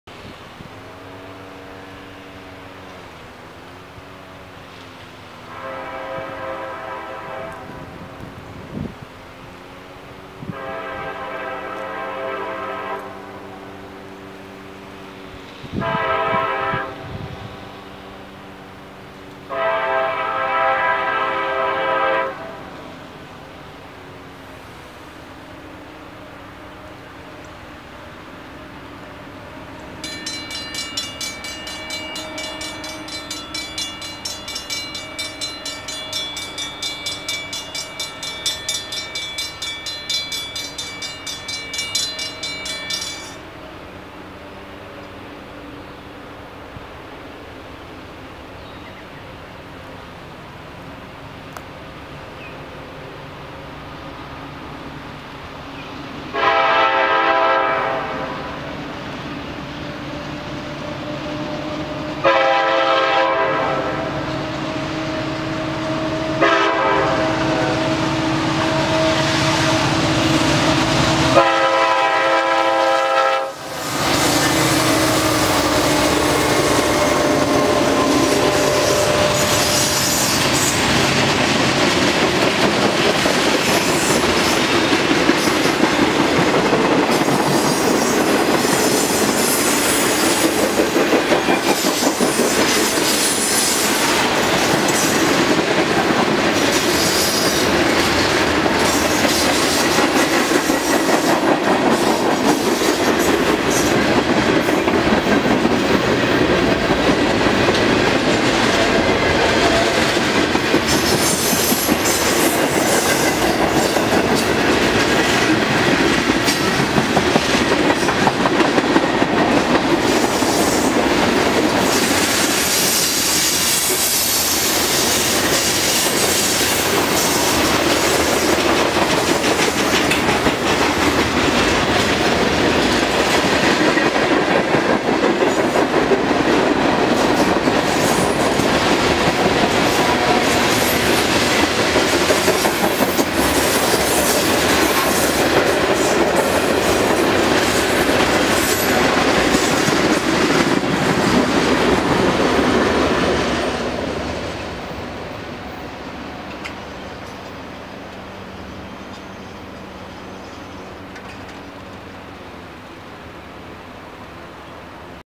NS SD60E leader hits the throttle while heading east @ Millbury w/ NS 14N + JD Tractors
NS 14N: NS 7005 [ex-HLCX/UP/C&NW] (SD60E) & NS 9900 (C44-9W). 60 Rail Cars. Shot in Millbury, OH on the NS Chicago Line. Filmed on 5/13/17.